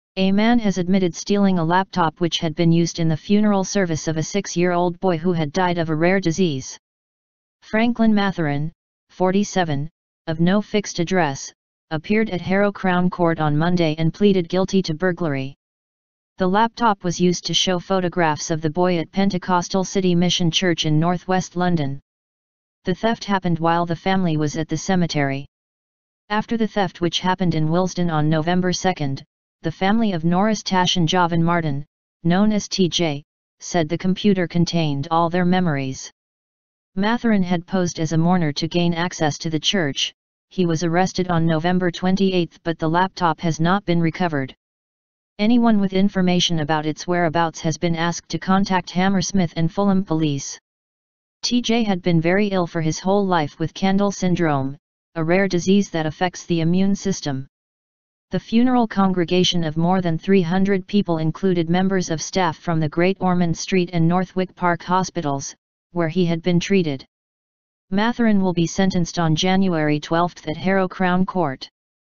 Man admits stealing laptop at funeral of boy of six - News from London
A man has admitted stealing a laptop which had been used in the funeral service of a six-year-old boy who had died of a rare disease.
Franklyn Mathurin, 47, of no fixed address, appeared at Harrow Crown Court on Monday and pleaded guilty to burglary.
The laptop was used to show photographs of the boy at Pentecostal City Mission Church in north-west London.
The theft happened while the family was at the cemetery.  After the theft which happened in Willesden on 2 November, the family of Norris Tashan-Javan Martin, known as TJ, said the computer contained "all their memories".
Mathurin had posed as a mourner to gain access to the church, he was arrested on 28 November but the laptop has not been recovered.
Anyone with information about its whereabouts has been asked to contact Hammersmith and Fulham Police.
TJ had been very ill for his whole life with Candle Syndrome, a rare disease that affects the immune system.
The funeral congregation of more than 300 people included members of staff from the Great Ormond Street and Northwick Park hospitals, where he had been treated.
Mathurin will be sentenced on 12 January at Harrow Crown Court.